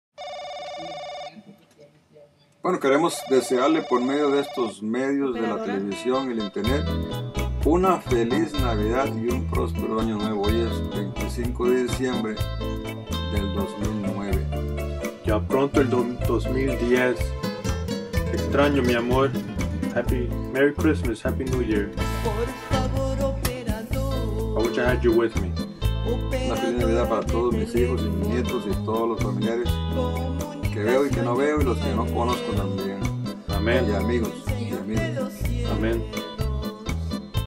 RICARDO GONZALEZ
SALUDOS NAVIDAD PARA MI ESPOSA , LA FAMILIA CORDOBA, MI HIJO Y SAMMY.